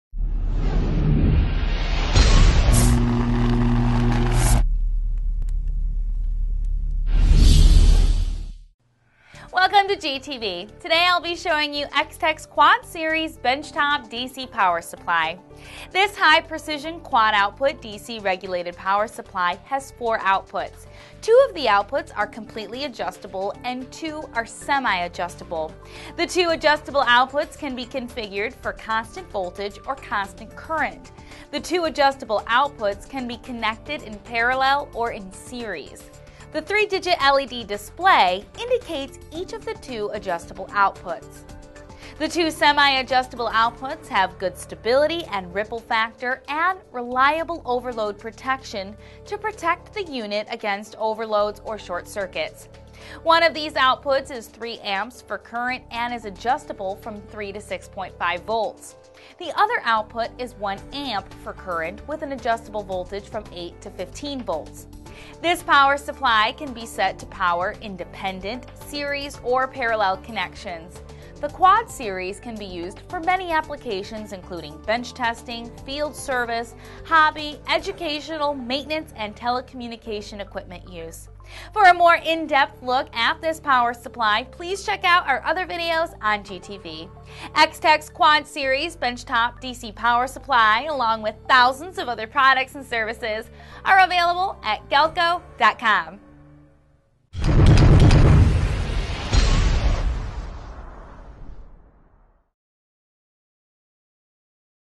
Extech Instruments Quad Series Benchtop DC Power Supply
This video shows a brief overview of the Extech's Quad Series Benchtop DC Power Supplypresented by Katie Rydzewski for Galco TV. Buy the item featured in this video at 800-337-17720

The Model 382270 is a high-precision DC regulated power supply with four outputs. Of which, two are completely adjustable and two are semi-adjustable. The two adjustable outputs can be configured for constant voltage or constant current. The two adjustable outputs can be connected in parallel or in series. The 3 digit LED display indicates each of the two adjustable outputs.

Quad Series Features:
-    To 0 to 30V/0 to 5A Outputs
-    Constant Voltage and Constant Current Modes
-    Outputs up to 60V in Series Mode or 10A in Parallel Mode
-    Two Auxiliary Outputs with Fixed Current
-    Overload and Short Circuit Protection
-    Complete with Power Cord and 4 Pairs of Test Leads

VIDEO TRANSCRIPT:
Welcome to GTV!
Today I'll be showing you Extech's Quad Series Bench top DC Power Supply.
This High Precision Quad Output DC regulated Power Supply has four outputs. Two of the outputs are completely adjustable and two are semi-adjustable. The two adjustable outputs can be configured for constant voltage or constant current. The two adjustable outputs can be connected in parallel or in series. The 3 digit LED display indicates each of the two adjustable outputs.

The two semi-adjustable outputs have good stability and ripple factor, and reliable overload protection to protect the unit against overloads or short circuits. One of these outputs is three amps for current and is adjustable from 3 to 6.5 volts.  The other output is 1 amp for current with an adjustable voltage from 8 to 15 volts.

This power supply can be set to power independent, series or parallel connections.  The quad series can be used for many applications including bench testing, field service, hobby, educational, maintenance, and telecommunication equipment use.

Extech's Quad Series Bench top DC Power Supply, along with thousands of other products and services are available at galco.com.